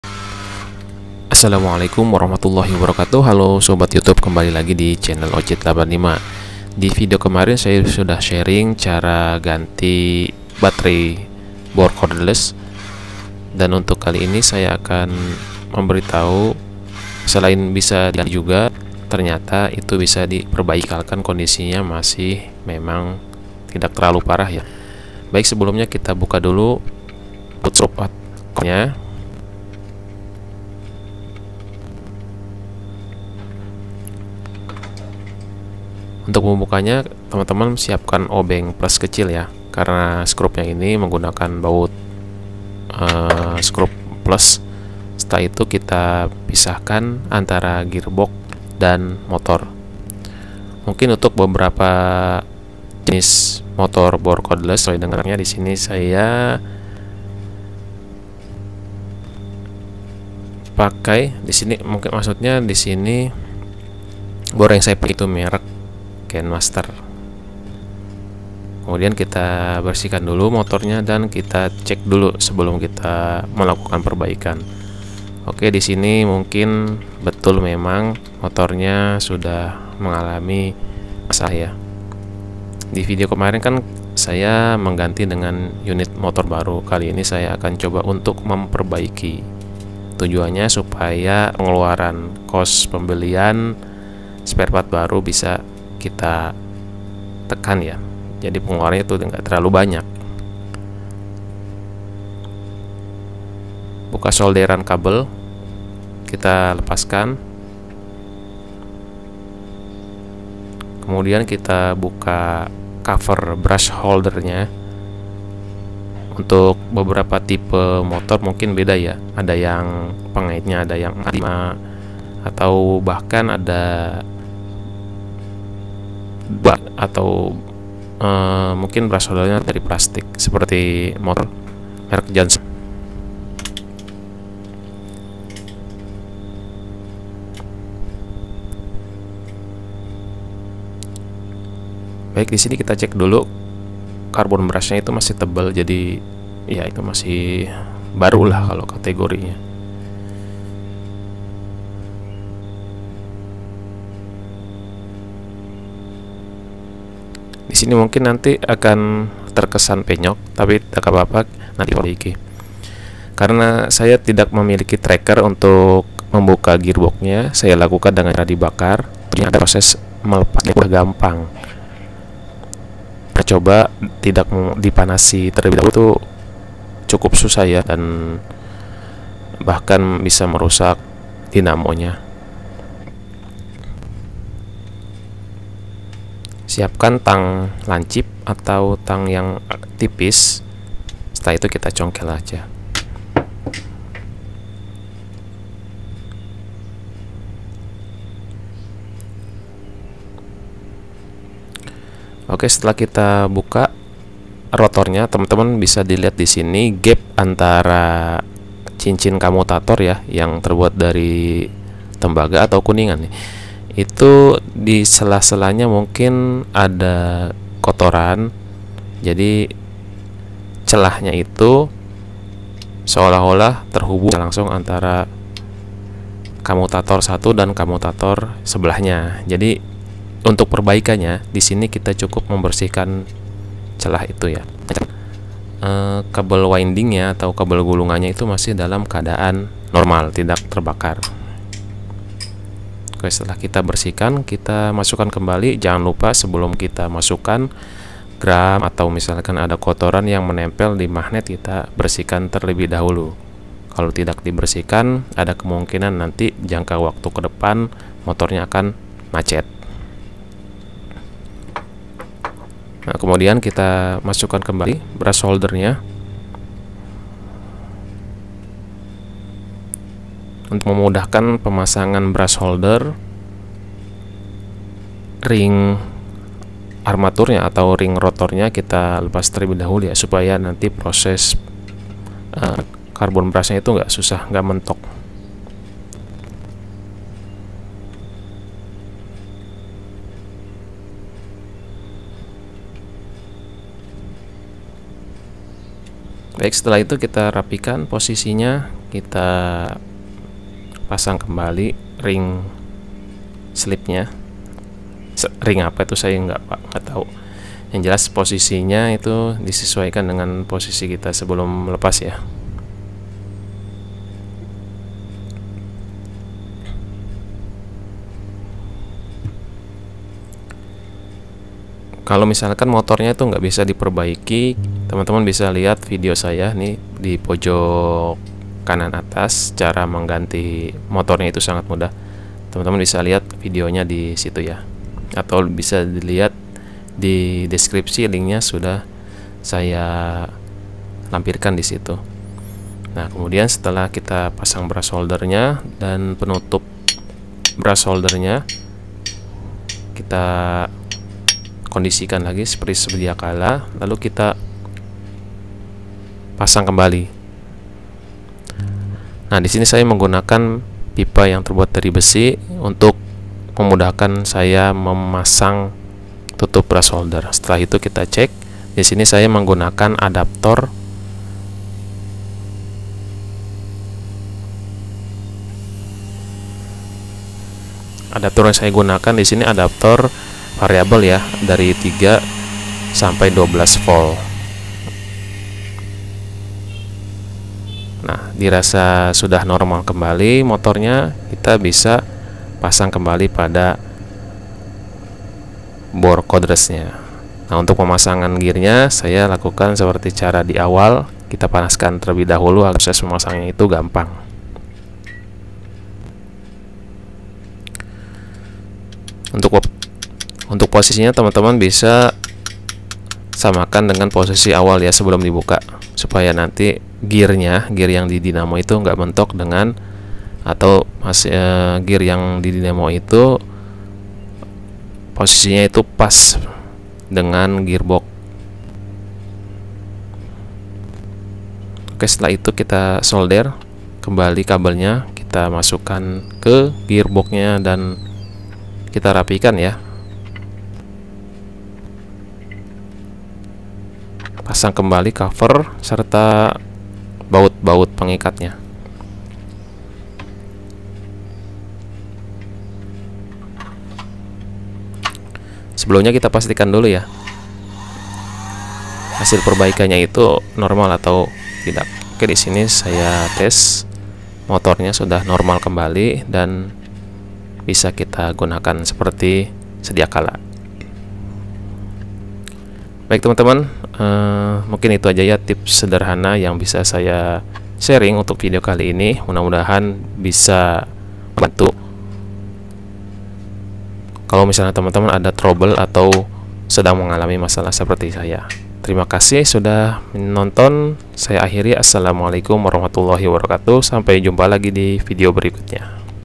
BOR BATERAI BERASAP
Assalamualaikum, video minggu lalu saya sudah memposting cara mengganti dinamo bor baterai ini link nya dimana kendala yang di hadapi yaitu bor baterai berasap, di video ini saya memberikan tips cara memperbaiki mor baterai berasap jadi untuk dinamonya tidak di ganti melainkan di perbaiki untuk menanggulangi bor baterai berasap.